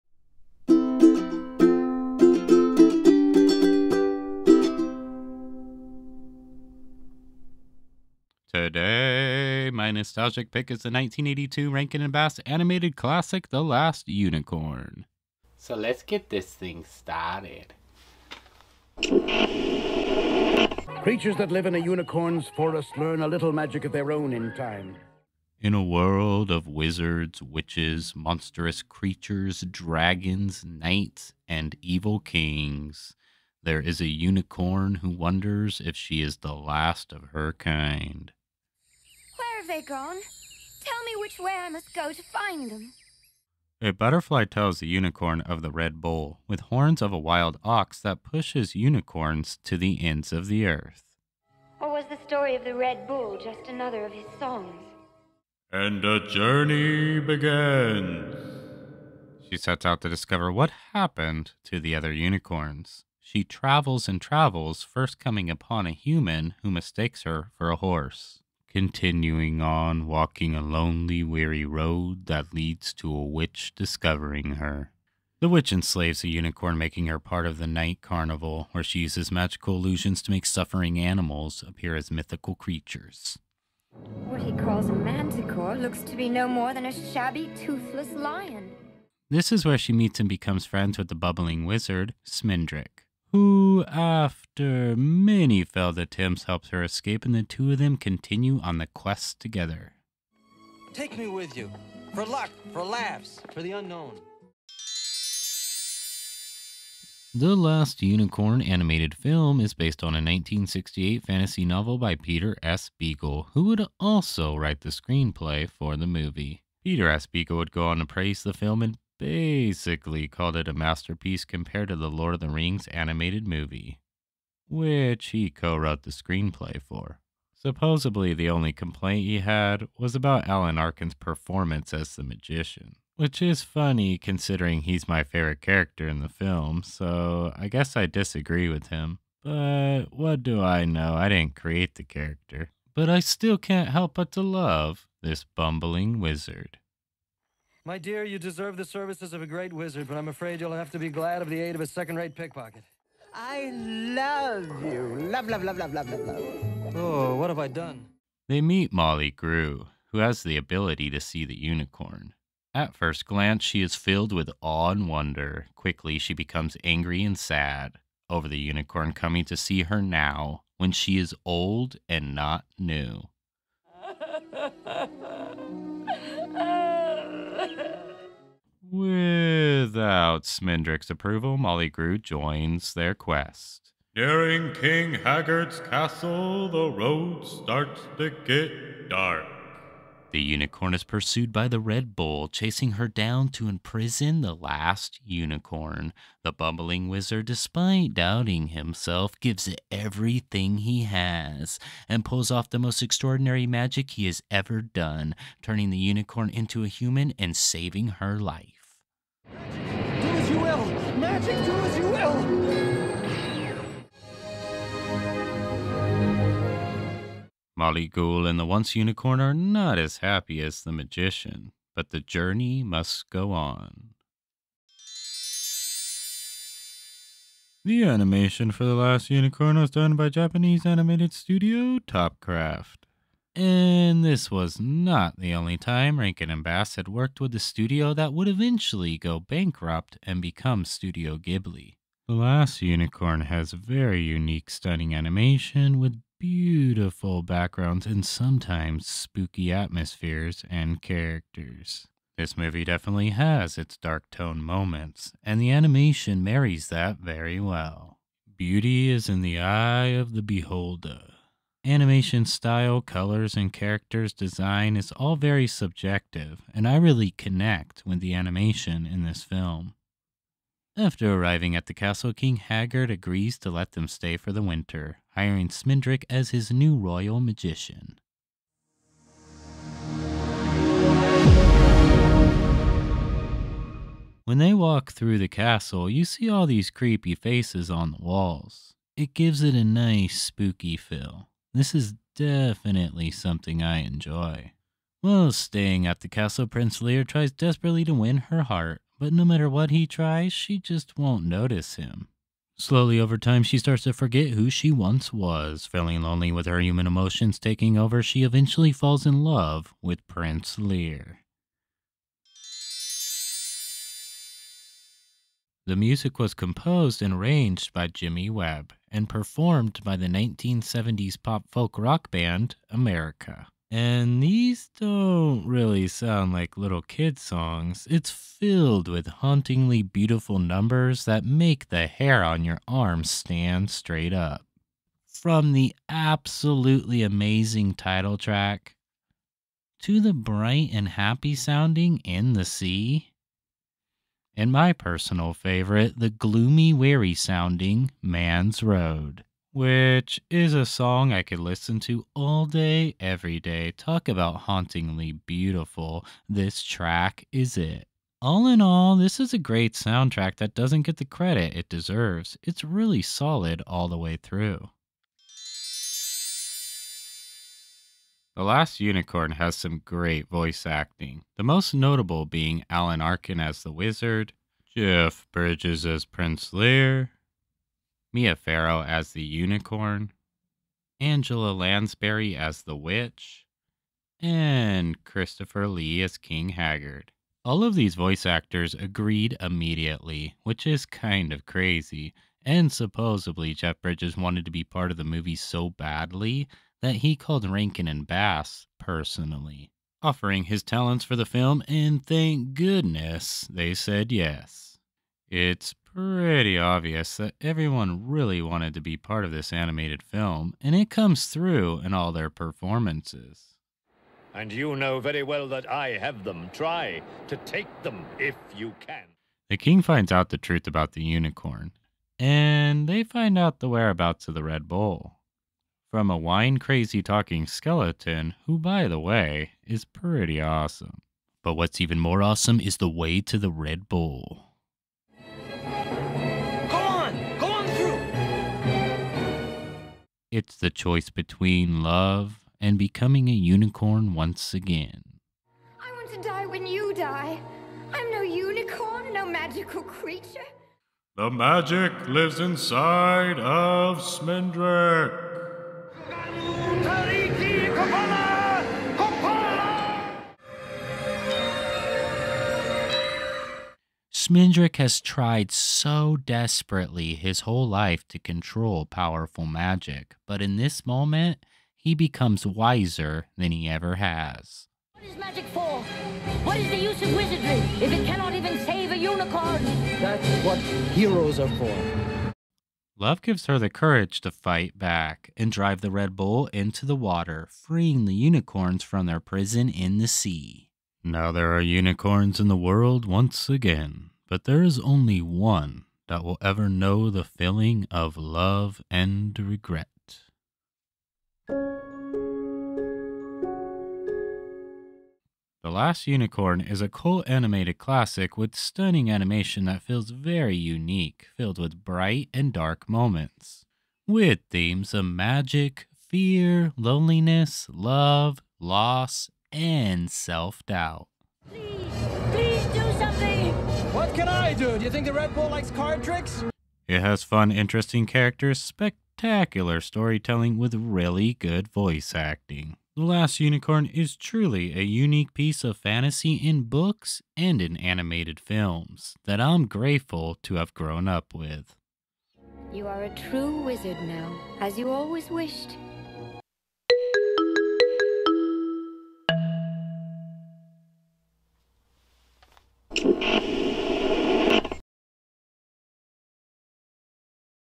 The Last Unicorn- Pick Nostalgic with Nostalgic Dom
Today My Nostalgic Pick is the animated Classic The Last Unicorn